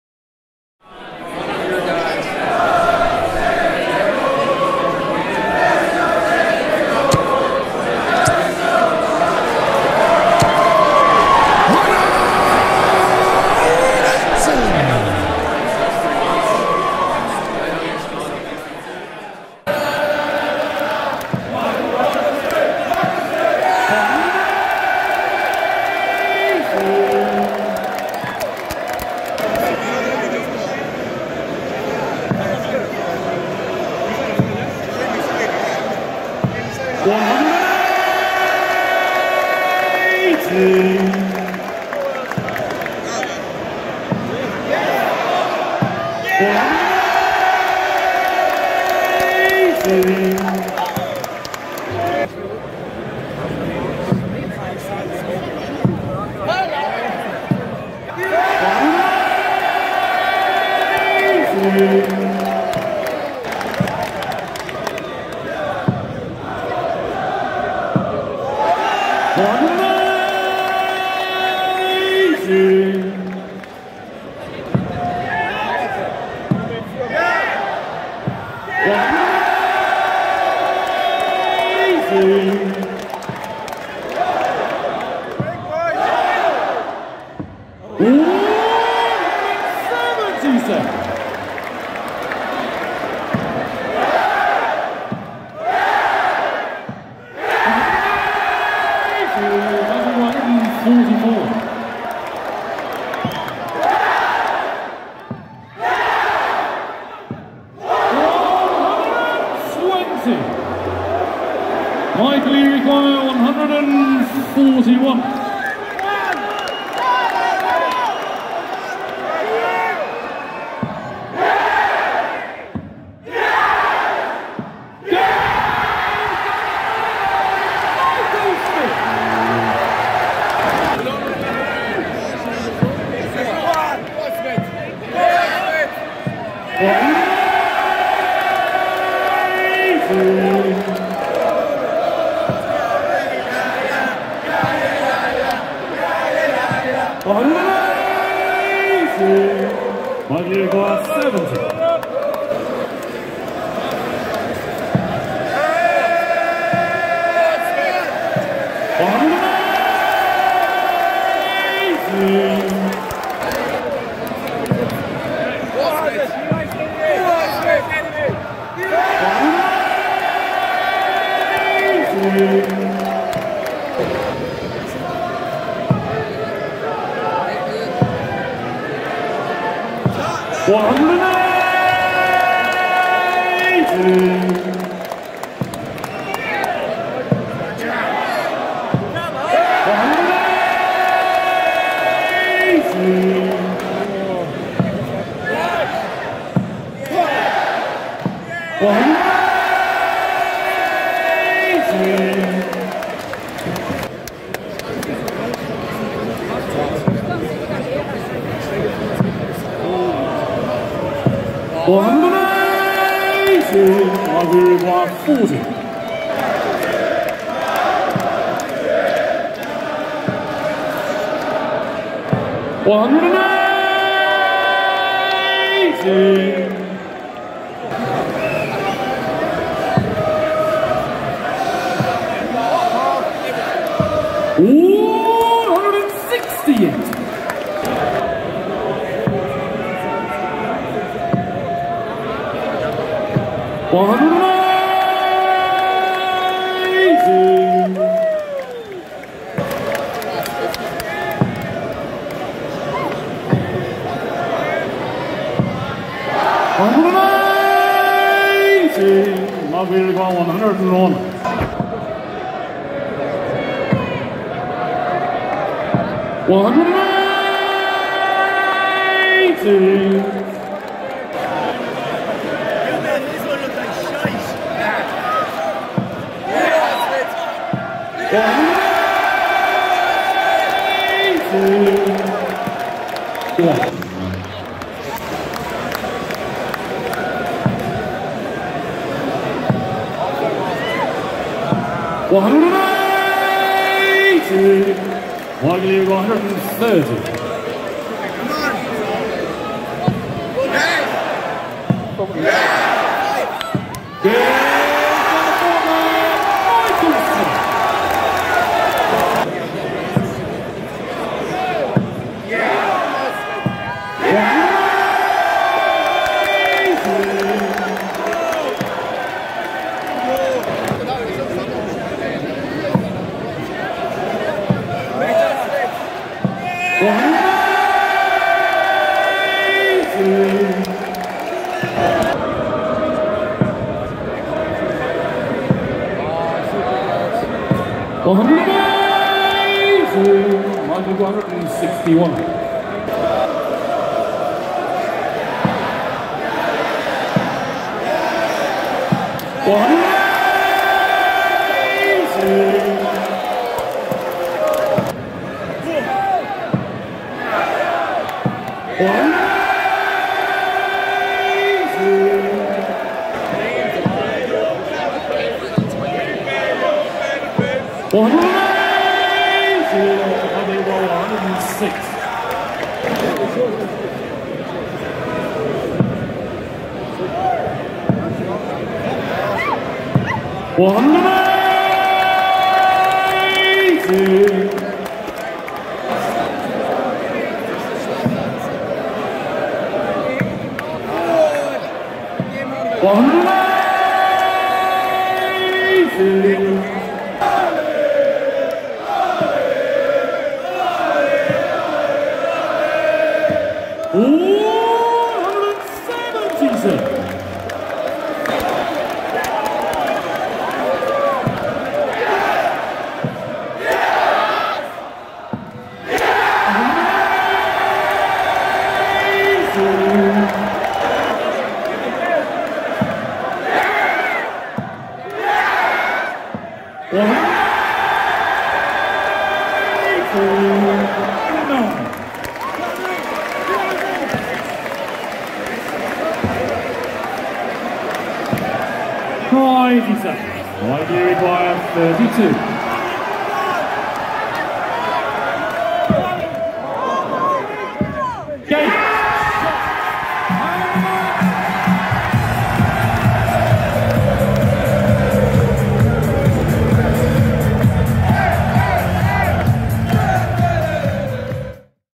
Ultimate Live 180s Compilation | MVG Vs. Smith | Cazoo World Championship of Darts Finals 2023
A 180s Compilation live Inside The Darts from the final at the Cazoo World Championships Alexandra Palace 2023. MVG vs. Michael Smith. Featuring the
"17 Dart Perfect Leg" between both players